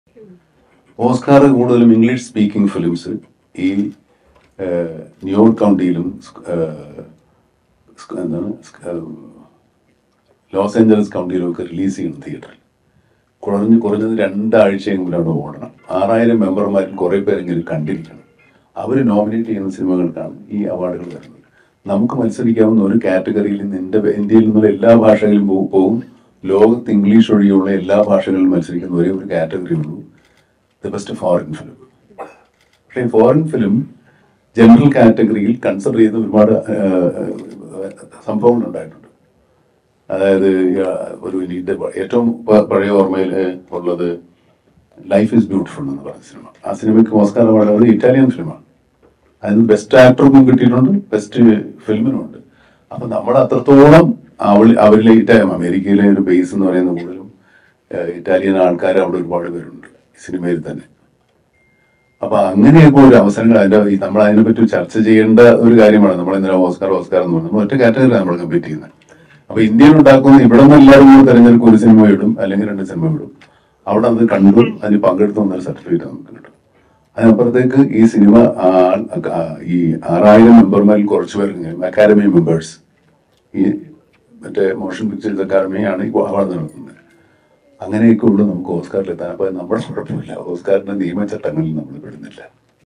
ഓസ്‌കാറിന്റെ നിയമചട്ടങ്ങളിലൊന്നും നമ്മൾ വരുന്നില്ല‌ ‌‌‌| Mammootty ‌| Christopher
ഓസ്‌കാറിന്റെ നിയമചട്ടങ്ങളിലൊന്നും നമ്മൾ വരുന്നില്ല, എന്നാൽ ചർച്ചകൾ നടക്കണം' മമ്മൂട്ടി , mammootty talking about Christopher